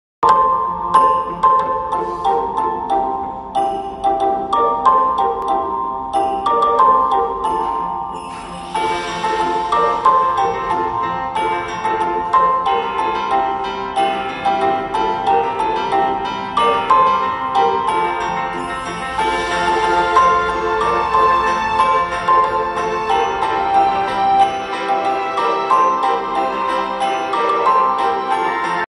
Li'l Pristinian Rockstars with Xmas vibes
Xmas vibes all around PristineEarlyYears & KeyStage1 The jazz is in the air as we await the RockNRoll party!

Li'l Pristinian Rockstars, are you ready?